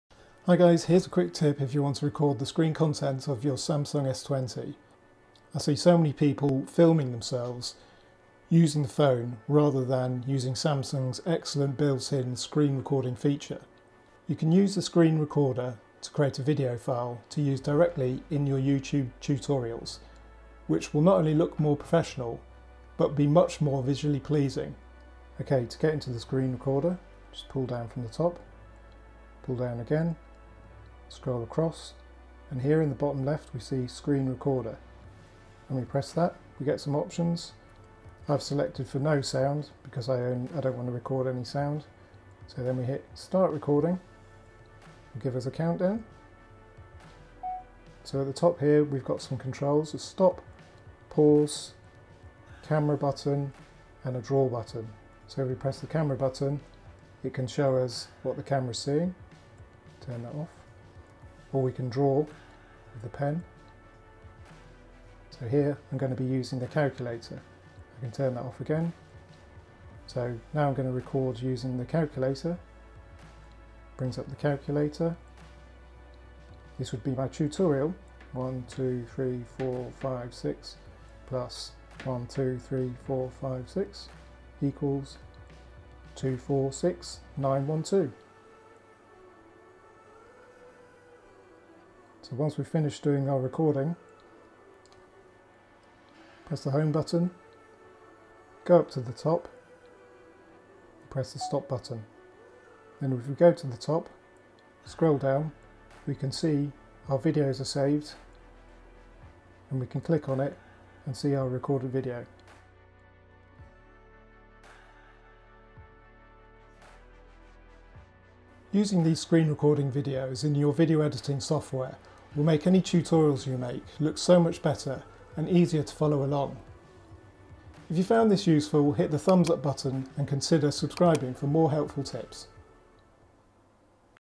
Samsung 20 Screen recording - How to record the screen on Galaxy S20, S20+ Galaxy S20 Ultra.
Quickly & easily record the screen on Samsung S20 models using the built in screen recording feature.